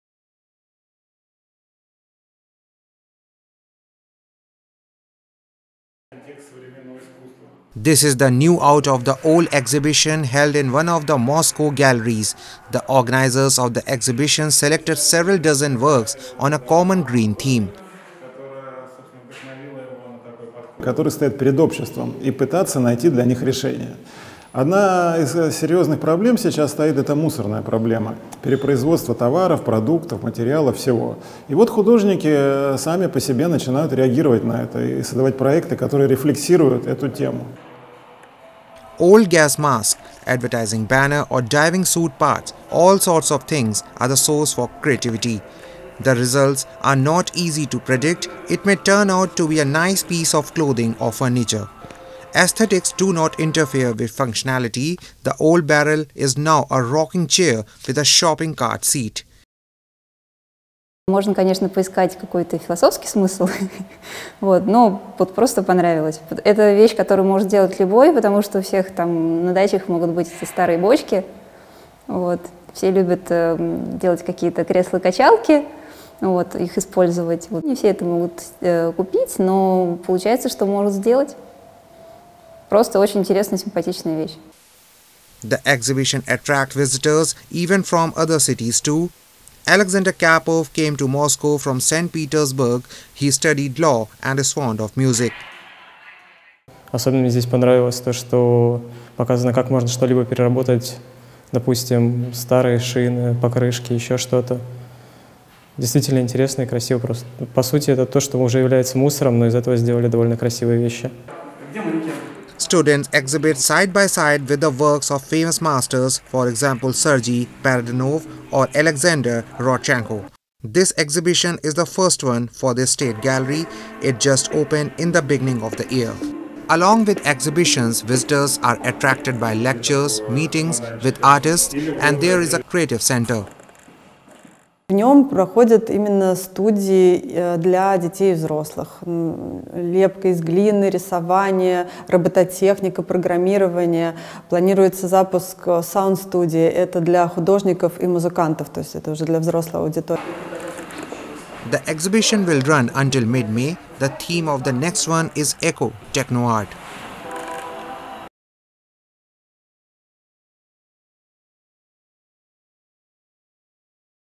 “New Out of the Old” Exhibition held in Moscow
Moscow (Russia), Apr 22, 2022, (ANI):  This is the “New Out of the Old” Exhibition held in one of the Moscow galleries.  The organisers of the exhibition selected several dozen works on a common green theme. Old gas mask, advertising banner or diving suit parts - all sorts of things are the source for creativity. The results are not easy to predict -it may turn out to be a new piece of clothing or furniture. Aesthetics do not interfere with functionality. The old barrel is now a rocking chair with a shopping cart seat. The exhibition attracts visitors even from other cities too.  Alexander Karpov came to Moscow from St. Petersburg. He studies law and is fond of music. Student exhibits side by side with the works of famous masters. For example - Sergei Paradzhanov or Alexander Rodchenko. This exhibition is the first one for this state gallery. It just opened in the beginning of the year. Along with exhibitions, visitors are attracted by lectures, meetings with artists, and there is a creative centre. The exhibition will run until mid-may. The theme of the next one is "eco techno art". (JA)